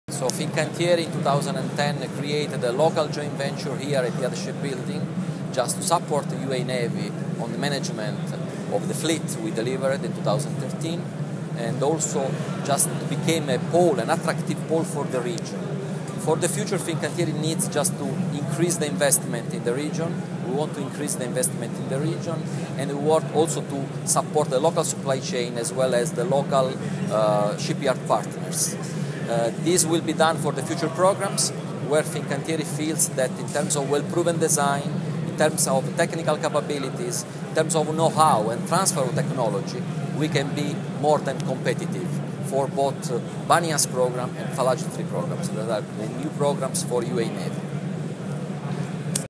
Achille Fulfaro, VP Marketing Development ME, Fincantieri commenting on the future plans of the comp
Achille Fulfaro, VP Marketing Development ME, Fincantieri commenting on the future plans of the company in UAE &  ME region